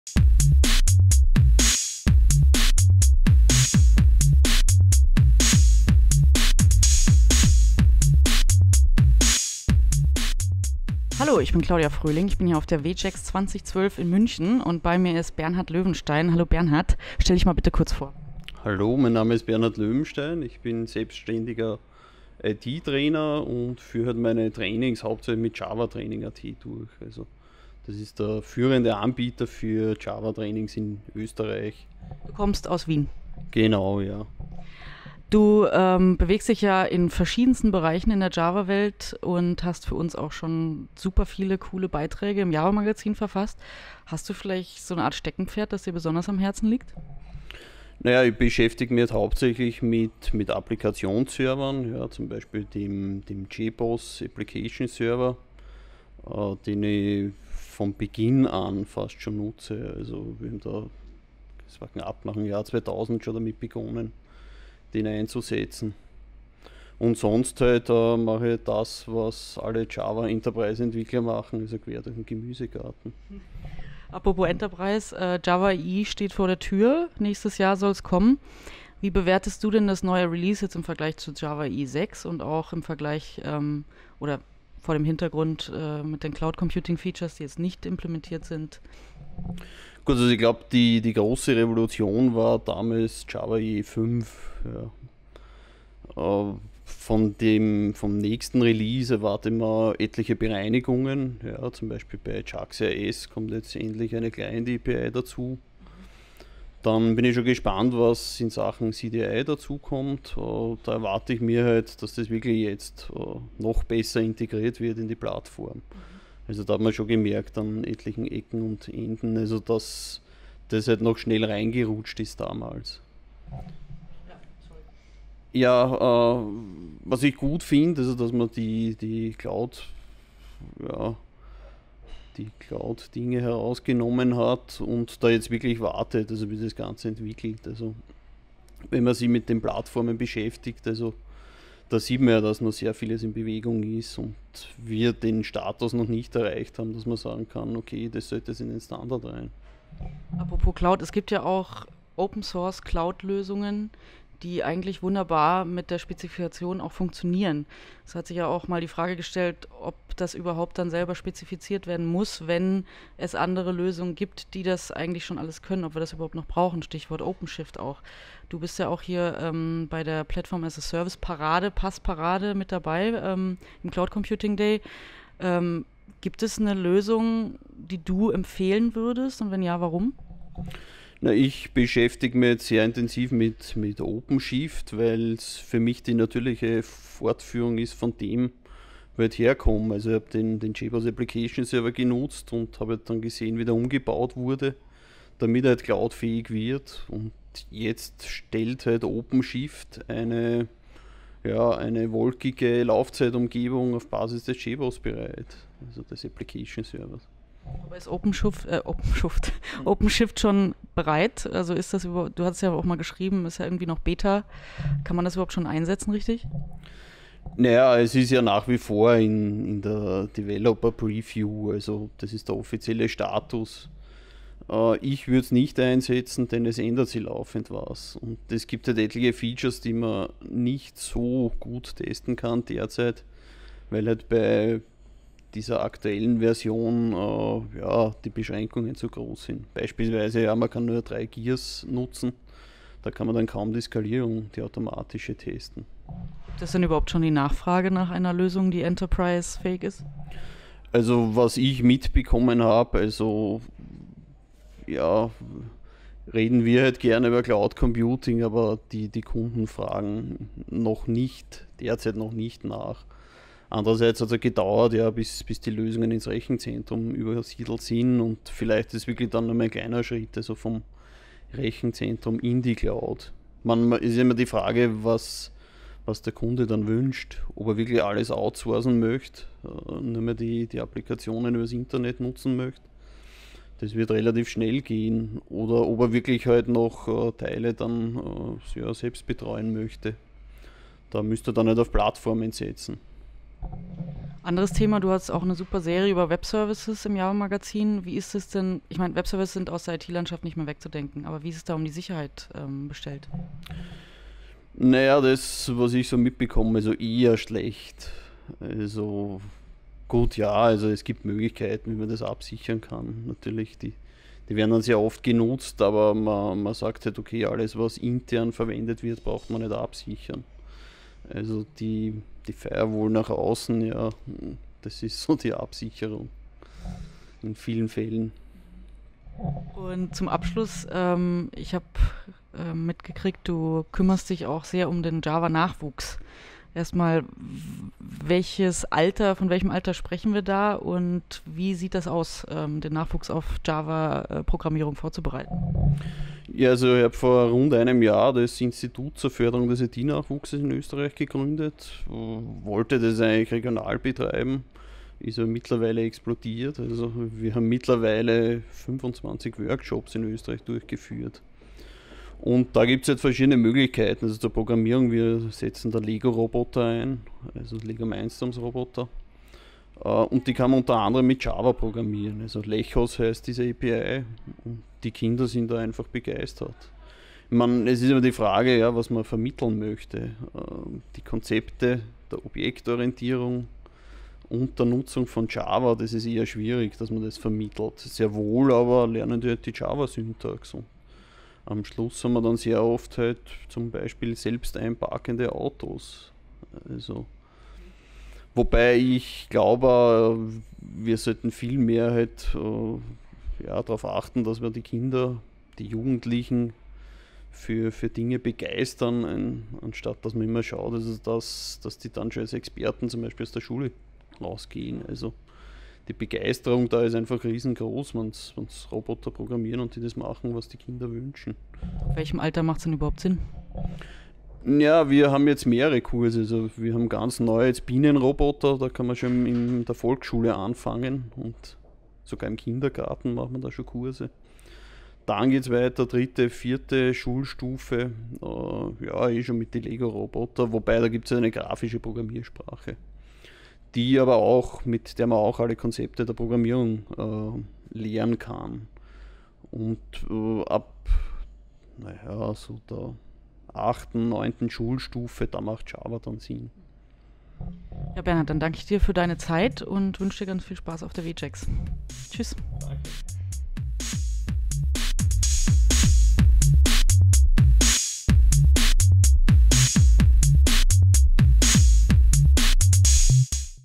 Interview: Java EE, OpenShift und die Java-Jugendarbeit
Wir sprachen auf der W-JAX 2012 mit Bernhard Löwenstein über Java EE 7, Platform as a Service und Bernhards Arbeit mit der Jugend.